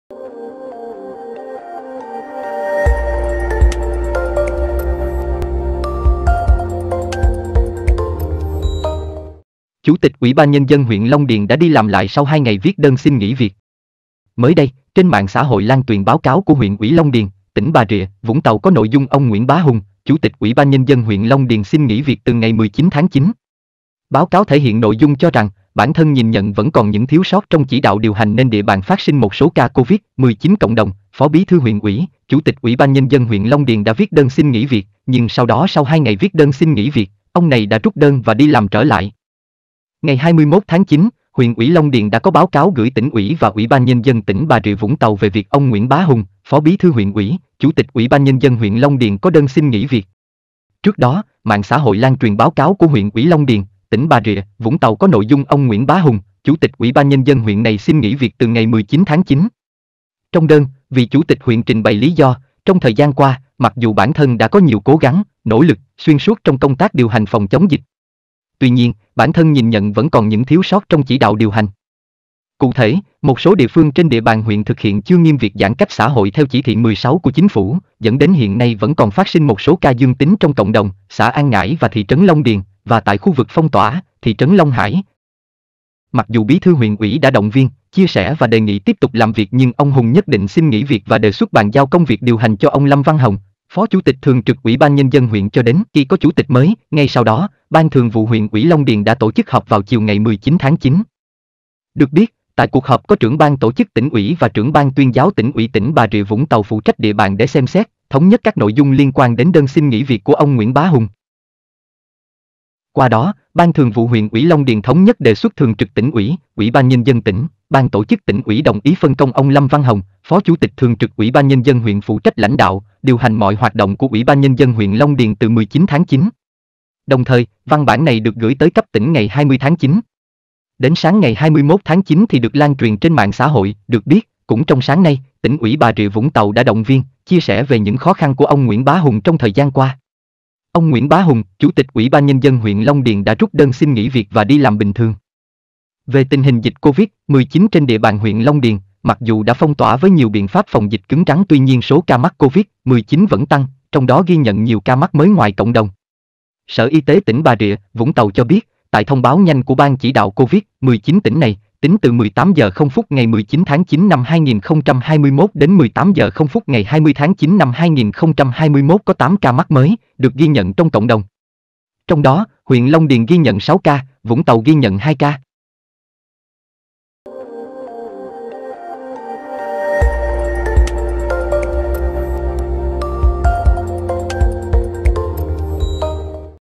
Chủ tịch UBND huyện Long Điền, Bà Rịa-Vũng Tàu đã đi làm lại sau 2 ngày viết đơn xin nghỉ việc.
Chủ tịch UBND huyện Long Điền đã viết đơn xin nghỉ việc, nhưng sau đó sau 2 ngày viết đơn xin nghỉ việc, ông này đã rút đơn và đi làm trở lại.